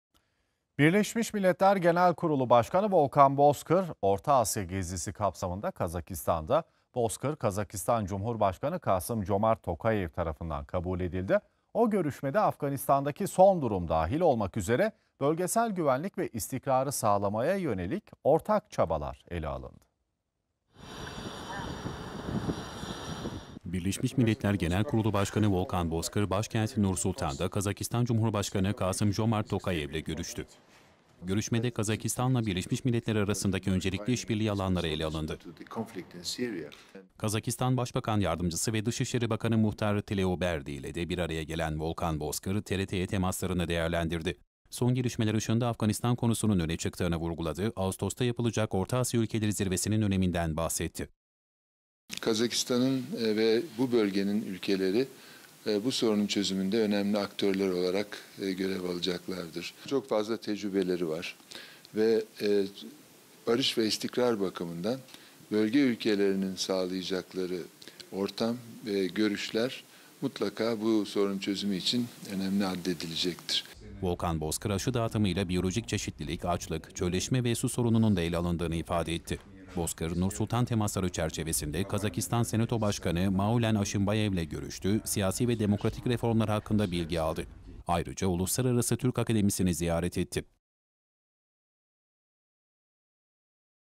BM-Kazakistan İlişkileri Görüşüldü-TRT AVAZ HABER-09.07.2021
Birleşmiş Milletler Genel Kurulu Başkanı Volkan Bozkır Orta Asya gezisi kapsamında Kazakistan’da resmi ziyaretlerde bulundu.

Bozkır, Kazakistan Cumhurbaşkanı Kasım-Jomart Tokayev tarafından kabul edildi.

Görüşmede Afganistan’daki son durum dahil olmak üzere  bölgesel güvenlik ve istikrarı sağlamaya yönelik ortak çabalara özel önem verildi.